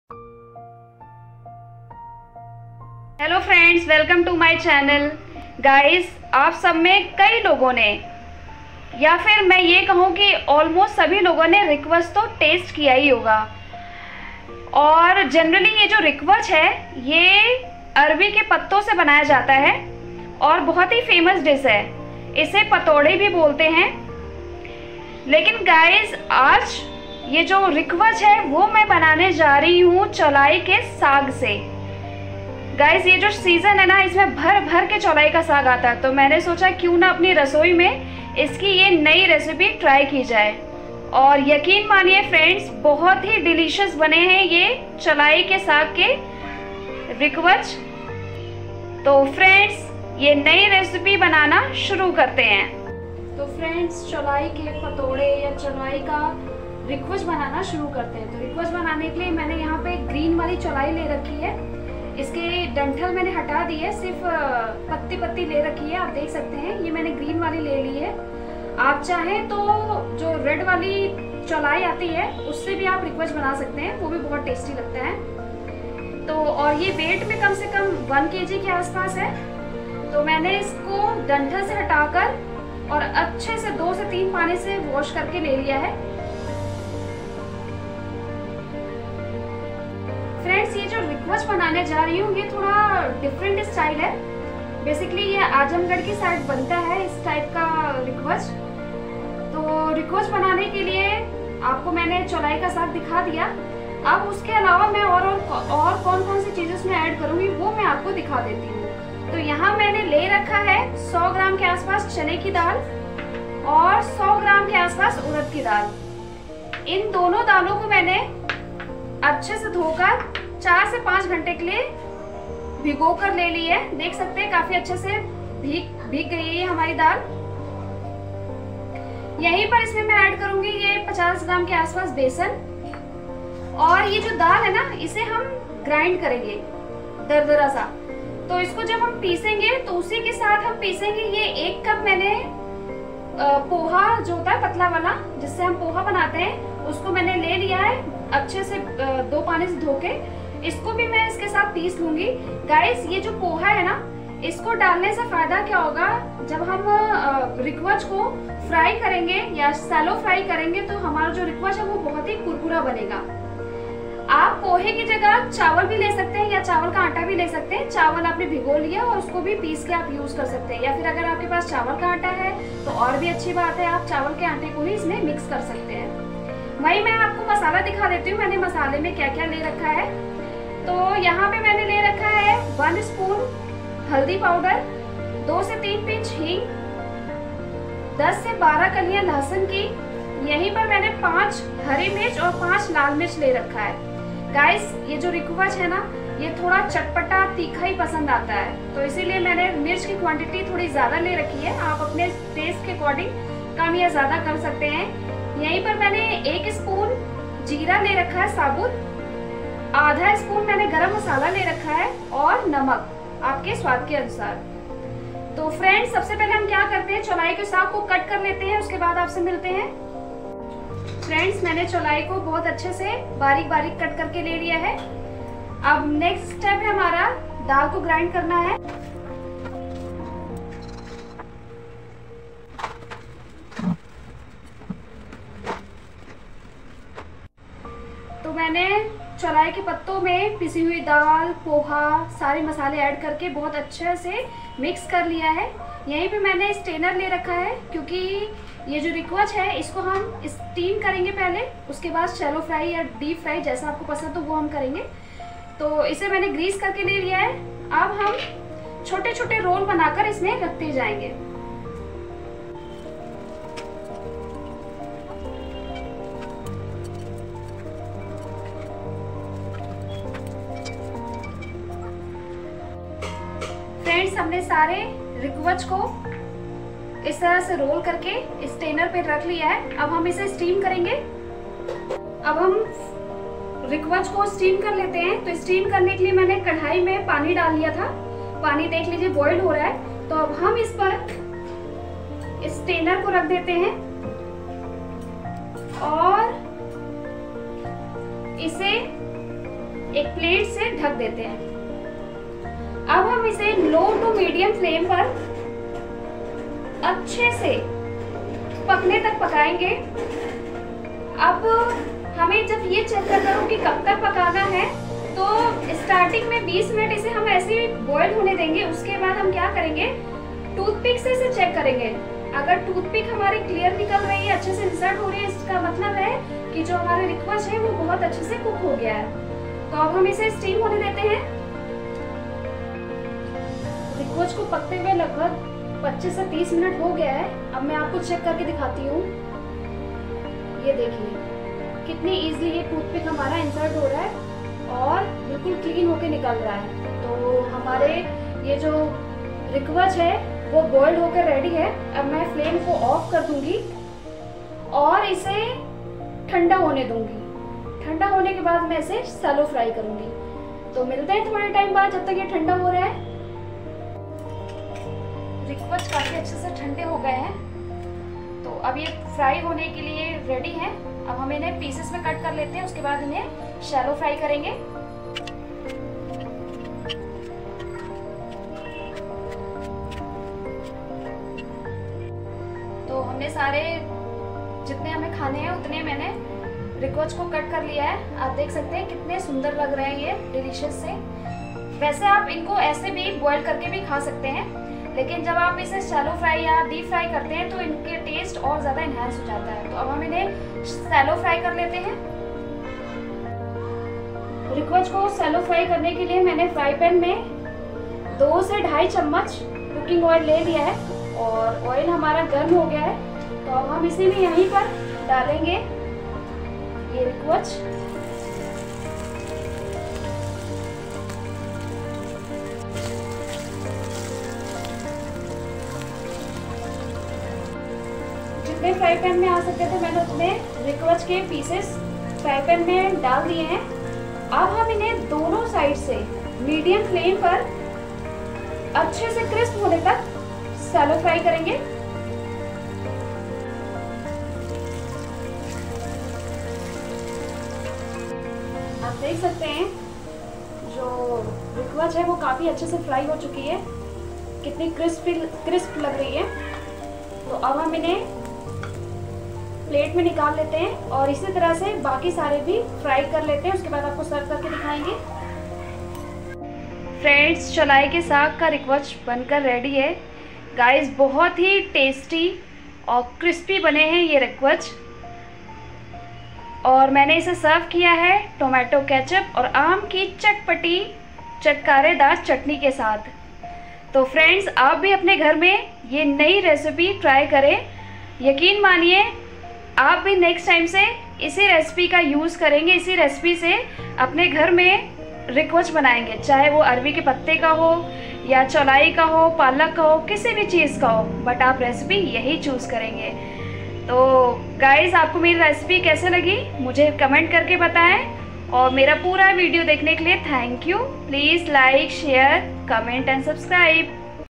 New recipe of chaulai saag.Chaulai saag ka Rikwach.UP style Rikwach recipe.chaulai ke patode.
Ingredients-
chaulai saag-1 kg
urad daal-100gm
chana dal-100gm
besan-50 gm
poha-1 Bowl, washed
red mirch-5
green chilli-5
garlic-10-12
hing-2 pinch

Recipe-
soak urad & chana dal for 4-5 hours
wash & chop chaulai saag
now grind daal,poha and all masala.
mix chaulai saag,daal paste & other remaining ingredients.
now make Rikwach and boil it for 25-30 minutes.
rest for 1-2 hours.now cut Rikwach and shallow fry it.
your delicious Rikwach are ready to eat.
Enjoy with mango chutney.